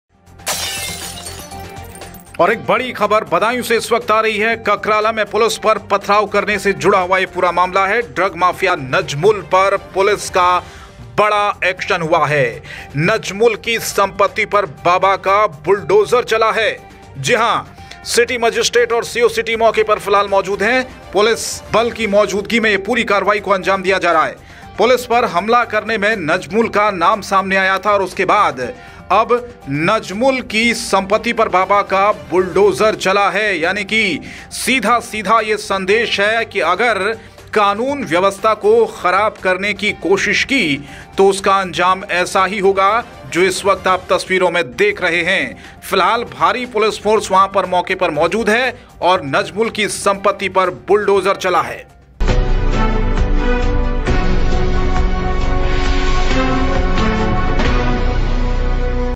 BREAKING NEWS: ड्रग माफिया नजमुल की संपत्ति पर चला बाबा का बुलडोजर
24x7 Hindi News Youtube Channel Jbt Live available on all social media platform's like Facebook, YouTube and Twitter.